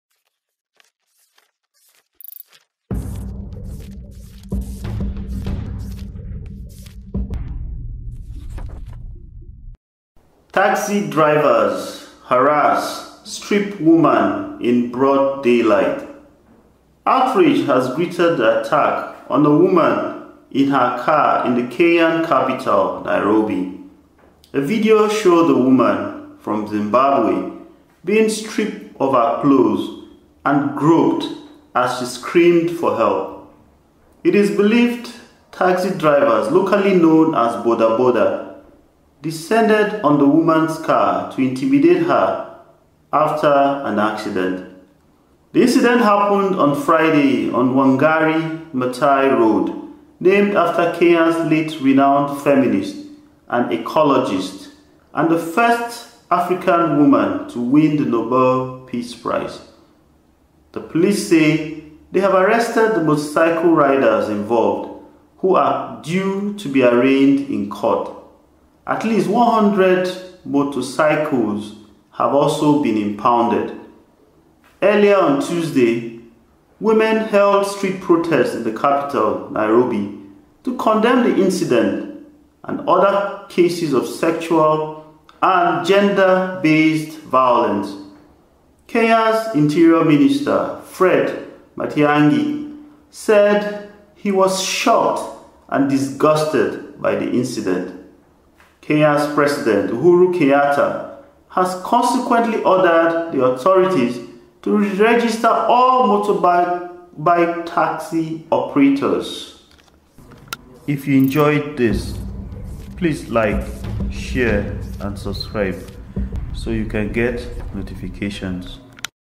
Taxi drivers harrass, strip woman in broad daylight
Kenya’s President Uhuru Kenyatta has ordered the authorities to re-register all motorbike taxi operators following the public outrage that greeted the attack on a woman in her car in the country’s capital, Nairobi.